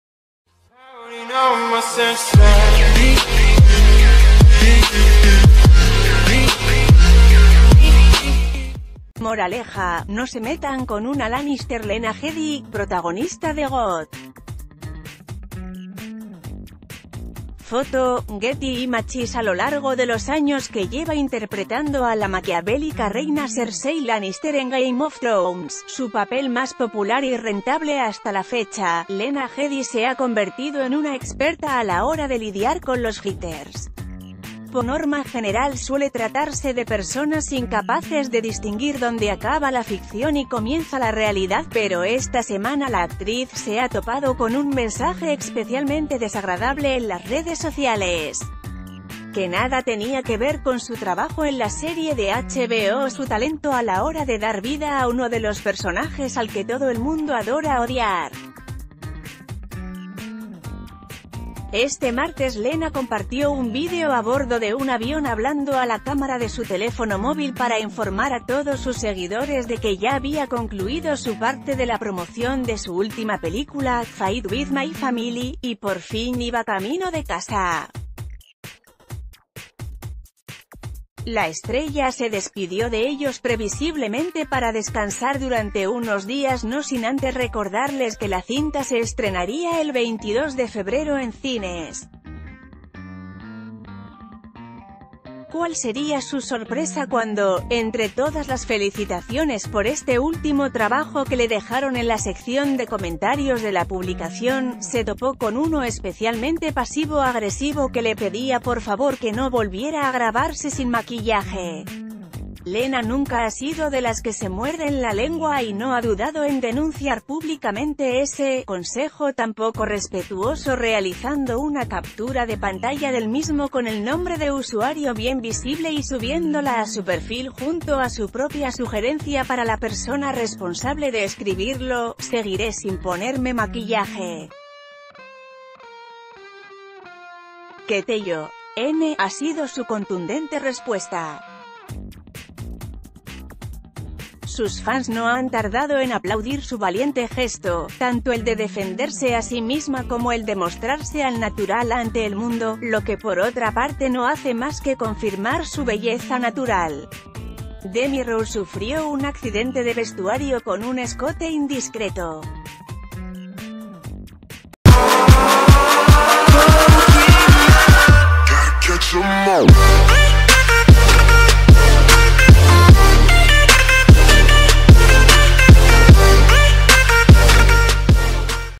La épica respuesta de “Cersei Lannister” de “Game of Thrones” a las críticas por no maquillarse
Moraleja: no se metan con una Lannister Lena Headey, protagonista de GoT. Foto: Getty Images A lo largo de los años que lleva interpretando a la maquiavélica reina Cersei Lannister en ‘Game of Thrones‘, su papel más popular y rentable hasta la fecha, Lena Headey se ha convertido en una experta a la ...